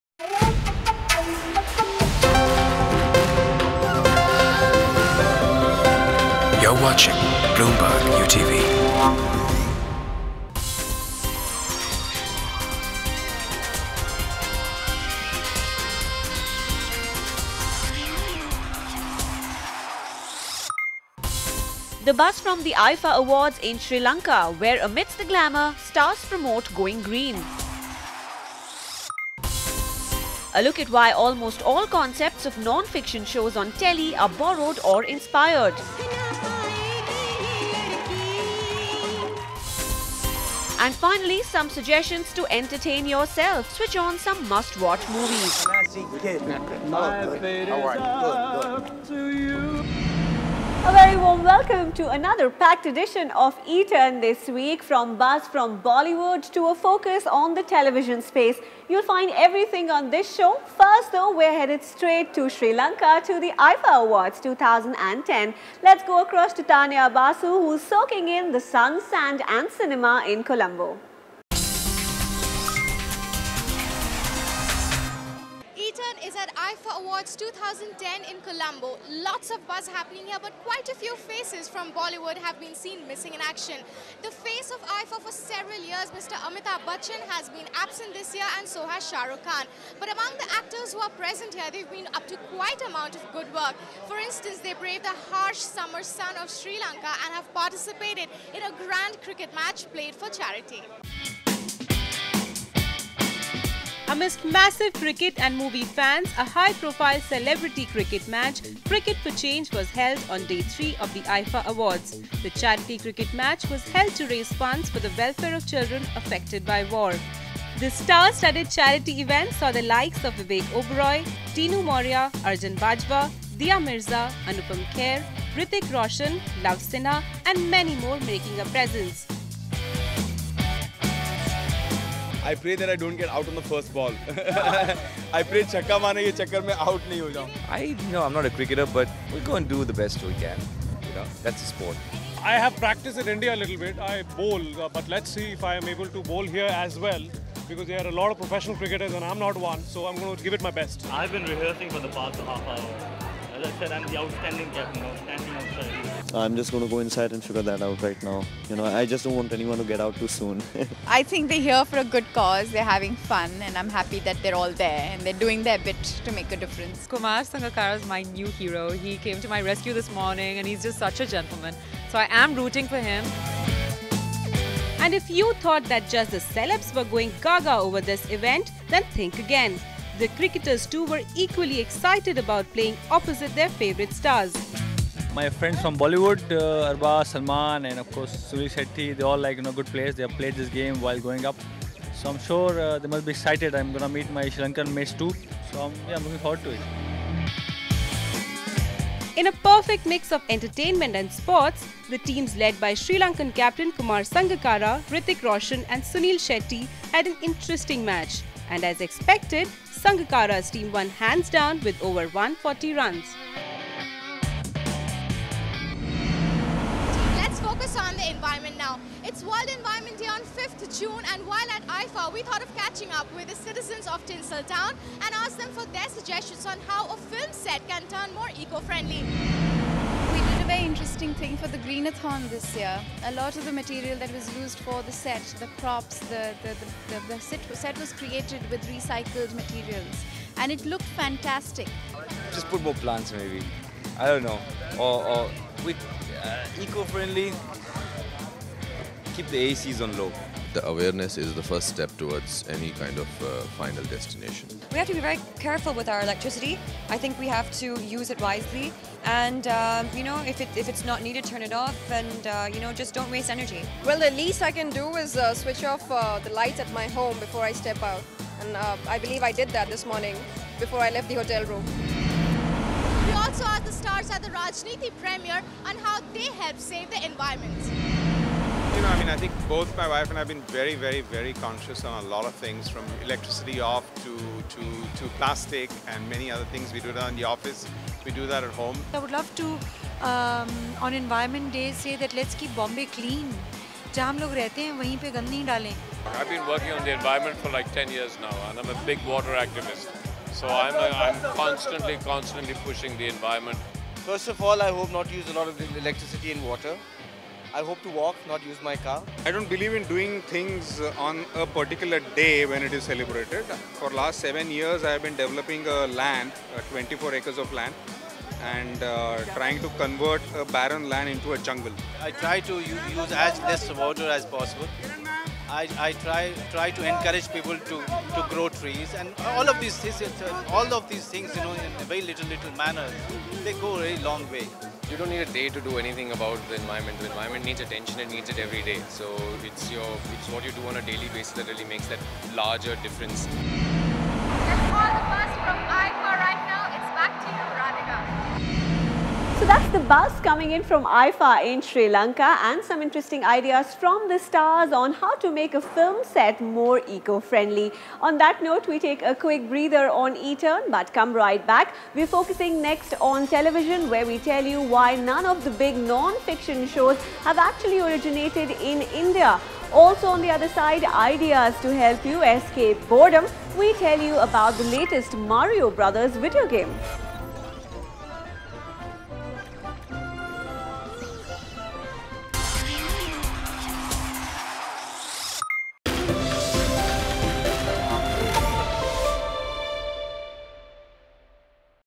E-Turn: Cricket stars at IIFA!
E-Turn brings you all the buzz from the IIFA awards in Sri Lanka.